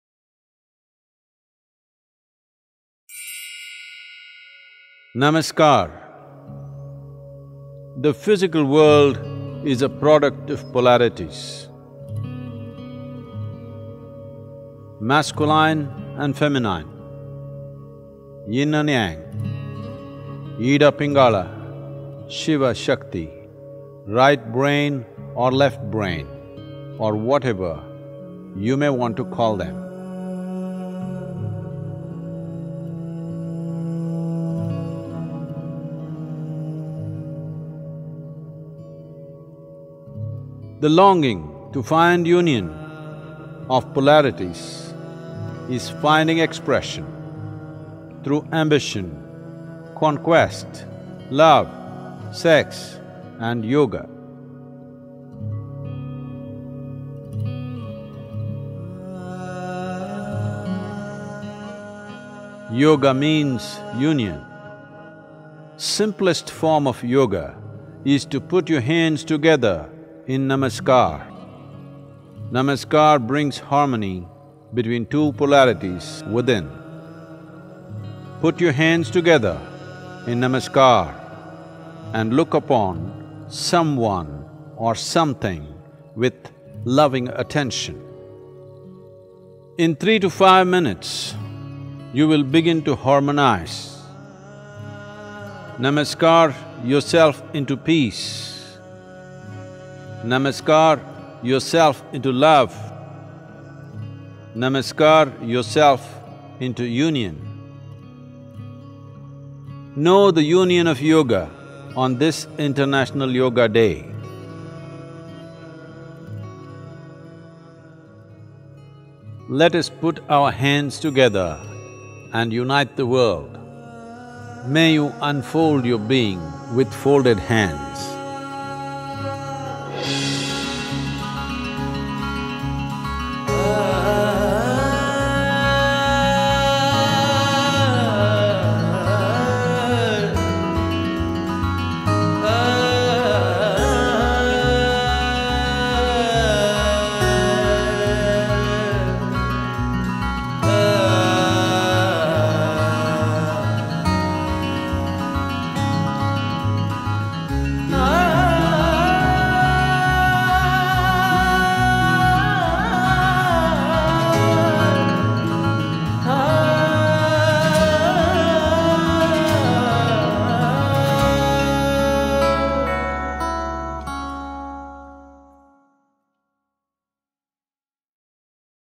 Namaskar - Yoga Para Todos | Sadhguru Português
Estamos felizes em lhe trazer este vídeo. Caracteriza rostos proeminentes em todo o mundo fazendo Namaskar, que é a forma mais simples de yoga. Ao unir as mãos, você pode trazer harmonia dentro de si e com o mundo ao seu redor.

* Caso a legenda não apareça automaticamente, ative-a no símbolo [cc]

Yoga e Meditação Online e Gratuita Guiada por Sadhguru (Inglês)